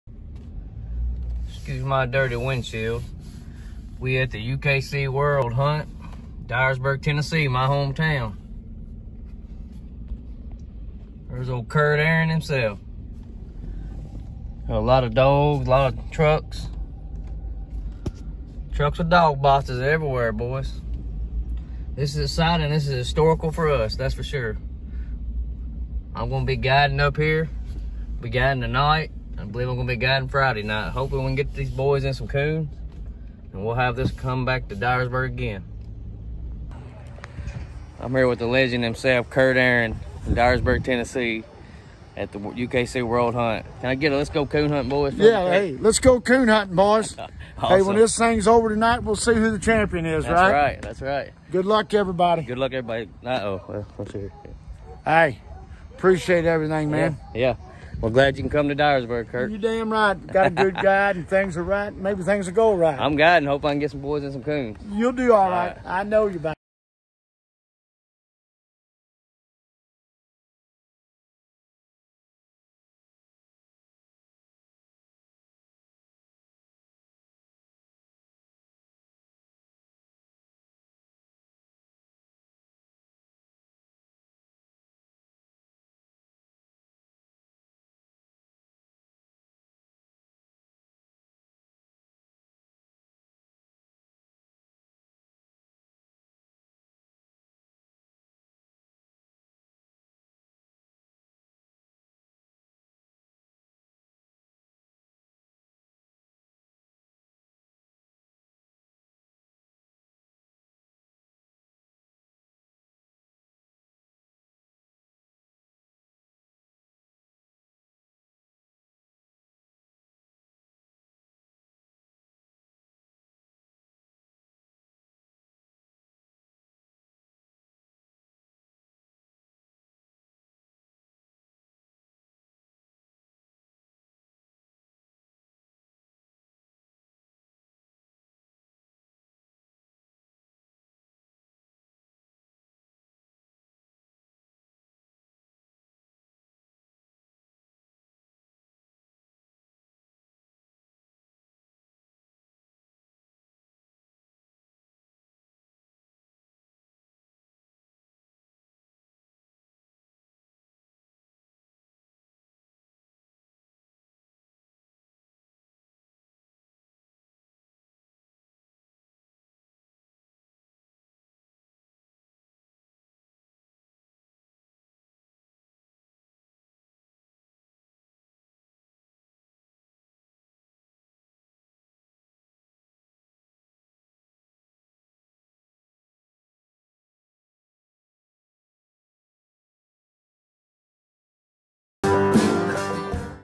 2022 Ukc world hunt
Little clips of the world hunt held from our local coon club in dyersburg, Tn